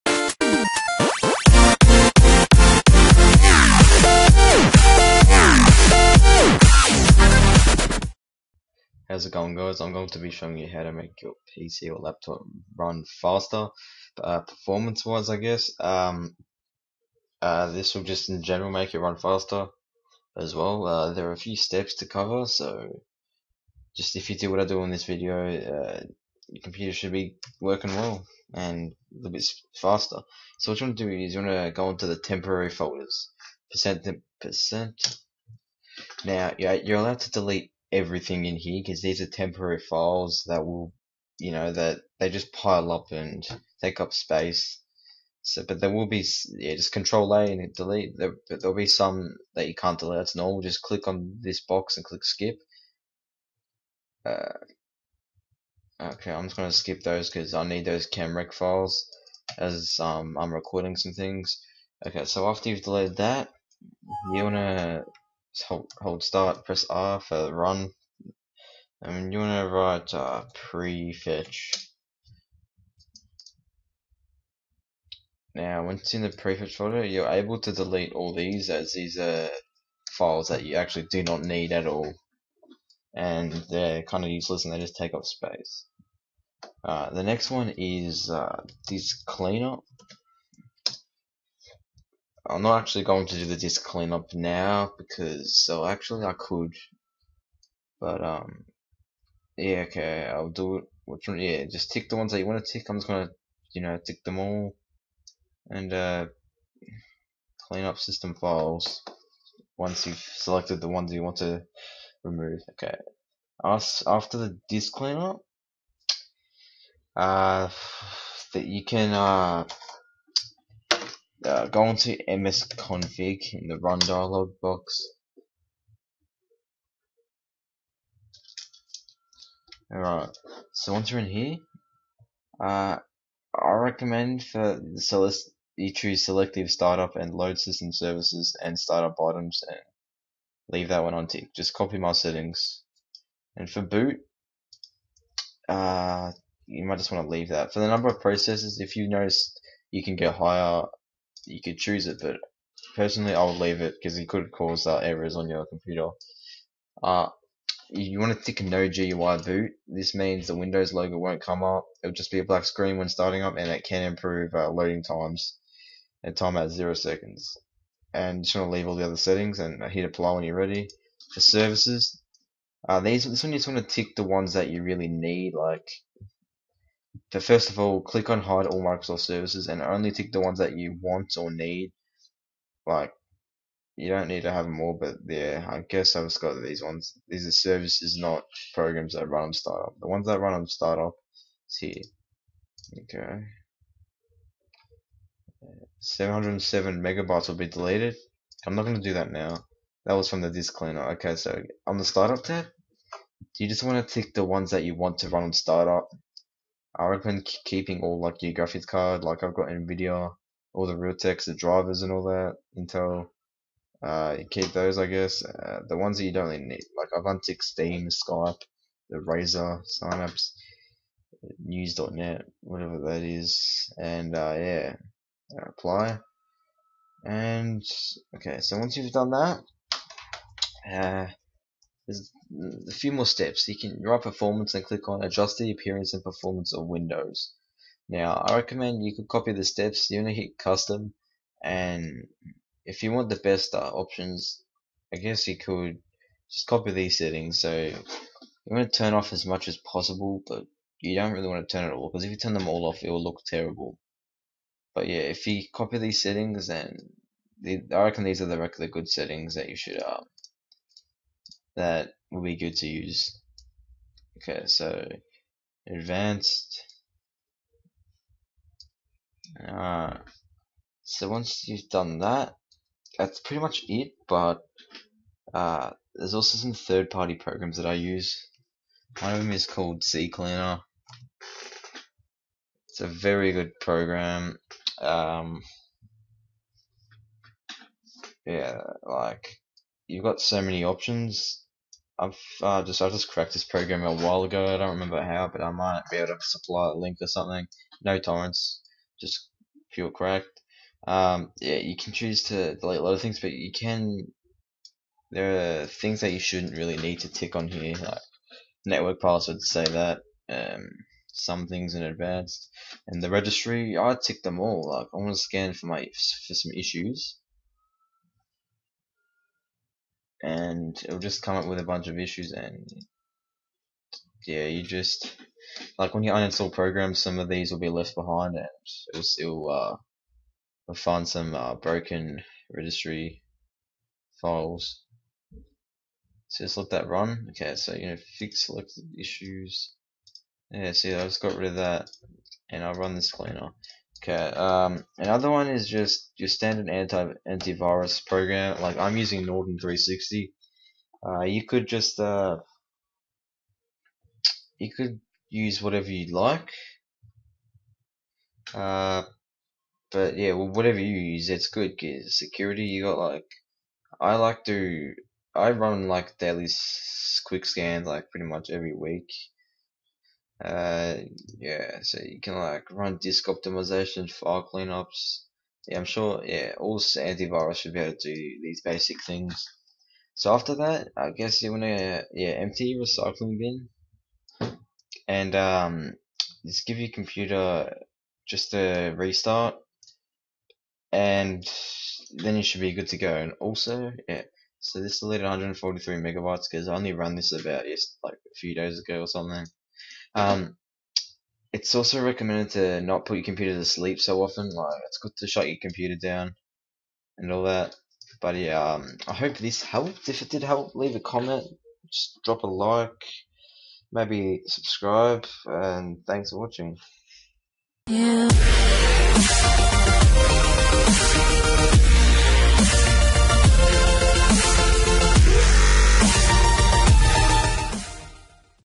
How To Make Your Computer Run Faster (EASY, NO PROGRAMS)
(This tutorial is noob friendly)

These are some simple steps to make your computer (PC/laptop) run faster performance wise! This will increase your average performance and you will definitely notice the difference! No programs are required as it is all in the operating system itself!
Watch the video and follow exactly what I do for the best possible performance for your computer!

TIP: Uninstall all of the programs that you don't need in the control panel. (Also helps to keep viruses away if you have a malicious program.)